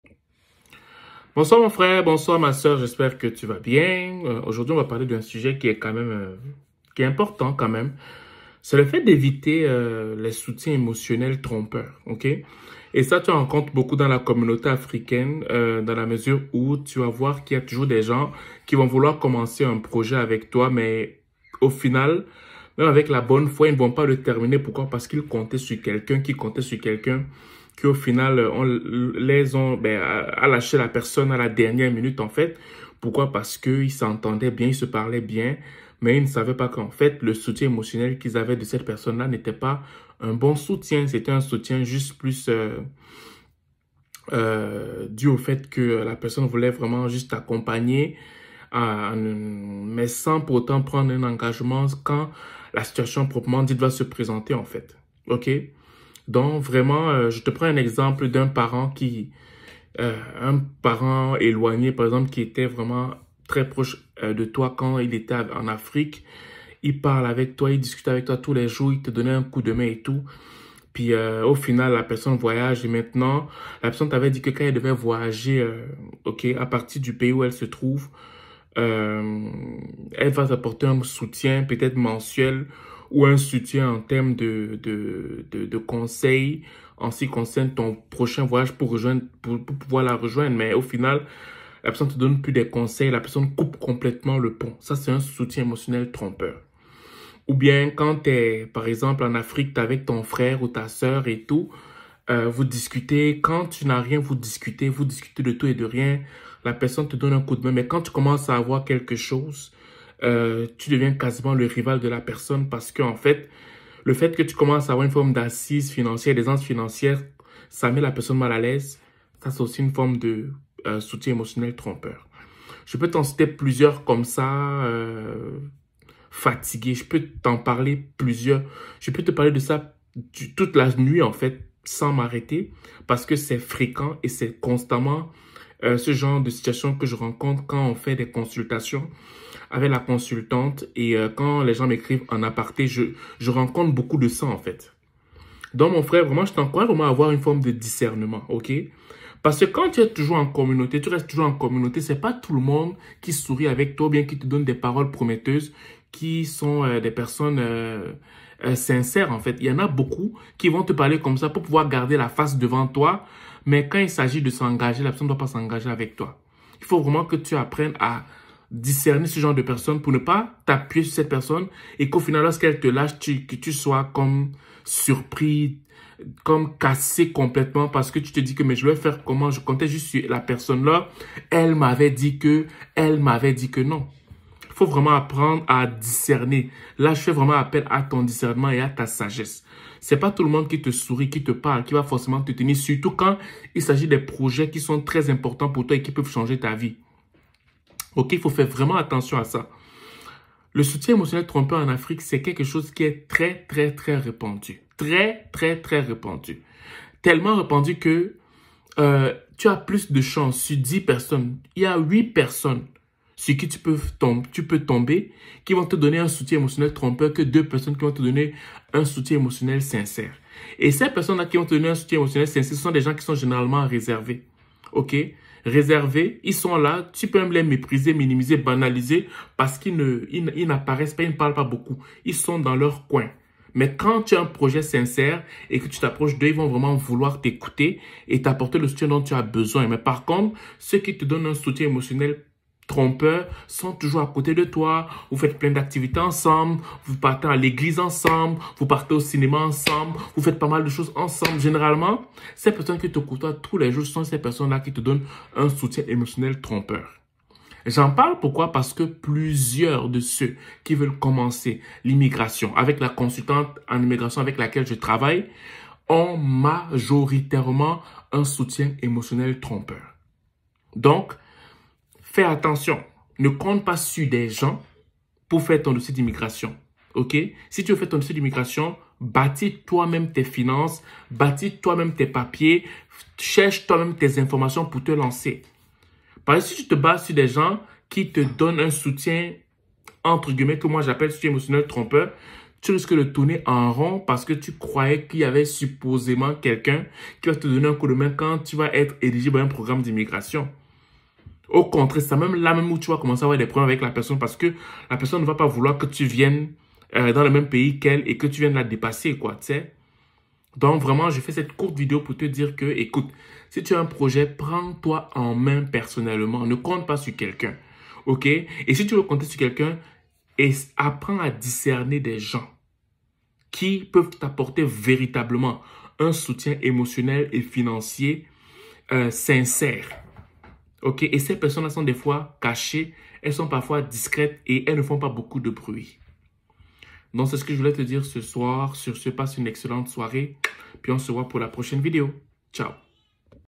Évite le soutien émotionnel trompeur dans tes projets de tes soit disant amis ou familles 🇨🇦
Pour savoir si tu es admissible à un programme d’immigration, enregistre-toi pour une consultation en immigration sur le lien suivant